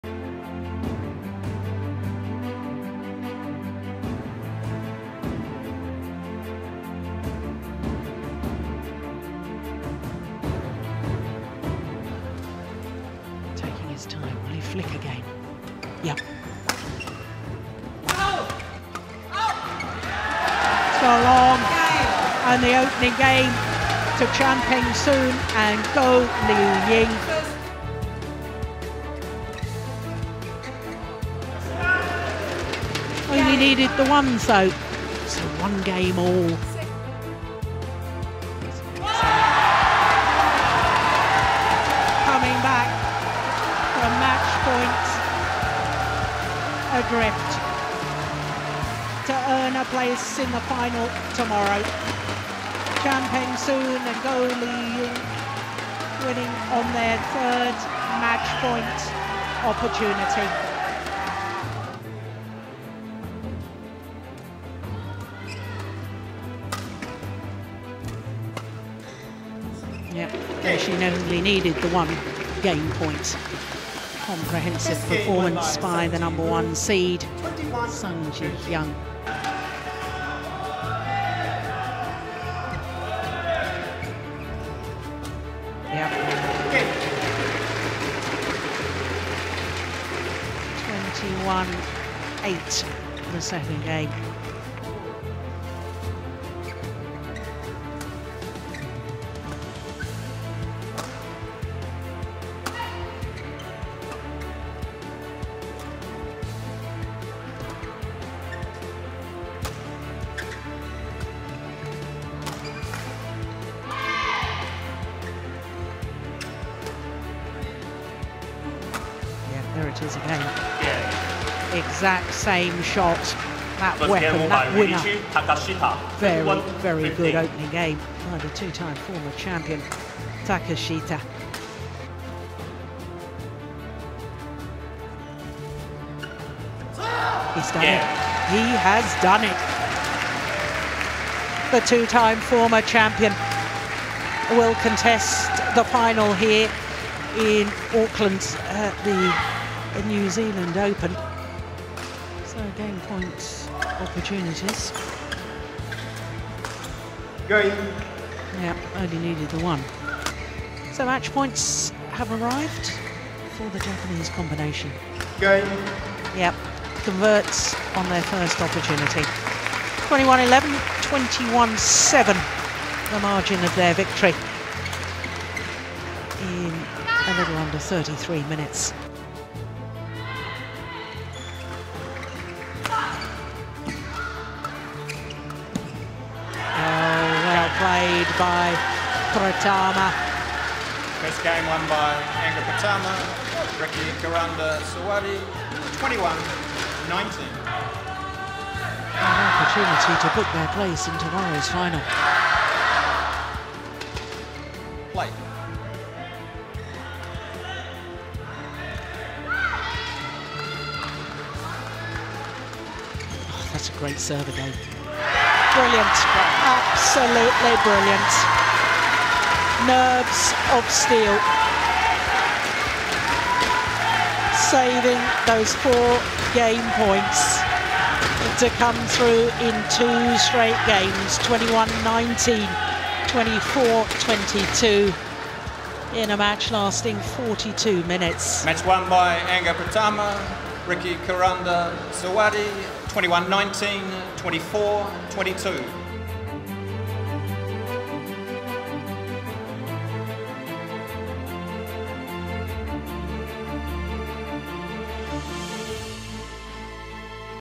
SKYCITY New Zealand Open 2016 | Badminton SF – Highlights
SKYCITY New Zealand Open 2016
Grand Prix Gold
Badminton
Semifinals
Highlights
Match 1 – XD | Ko Sung Hyun/Kim Ha Na vs Peng Soon Chan/Liu Ying Goh
Match 2 – WS | Sung Ji Hyun vs Chiang Mei Hui
Match 3 – MS | Riichi Takeshita vs Tien Minh Nguyen
Match 4 – WD | Yuki Fukushima/Sayaka Hirota vs Setyana Mapasa/Gronya Somerville
Match 5 – MD | Huang Kaixiang/Zheng Siwei vs Angga Pratama/Rcky Karanda Suwardi